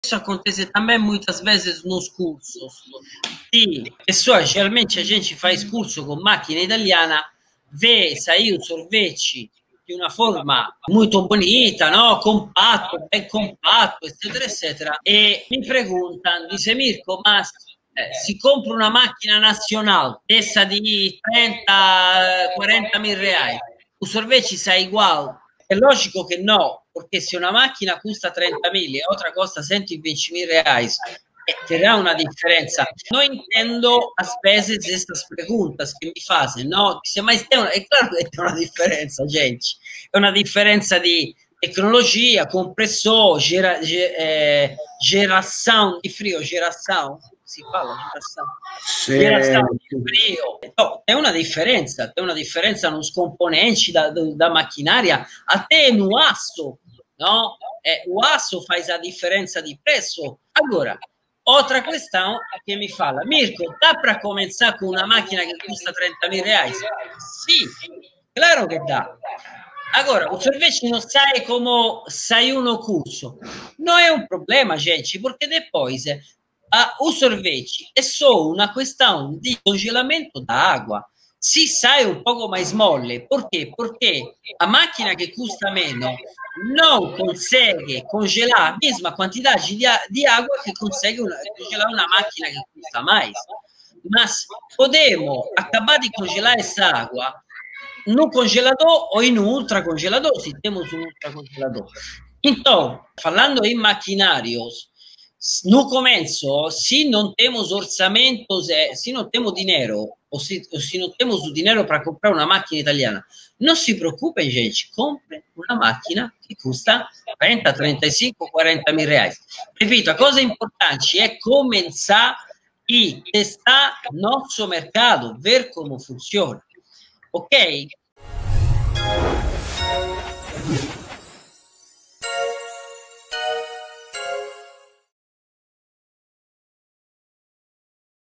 MÁQUINA DE SORVETE PARA QUEM ESTÁ COMEÇANDO
Quem não tem vontade de ter uma máquina de sorvete profissional incrível, não?

Mas será que tem diferença no resultado final do sorvete?

É muito comum ouvirmos em nossos cursos essa pergunta de "ficará igual se eu fizer em outra máquina?".

Dá uma conferida na resposta no vídeo de hoje! 😉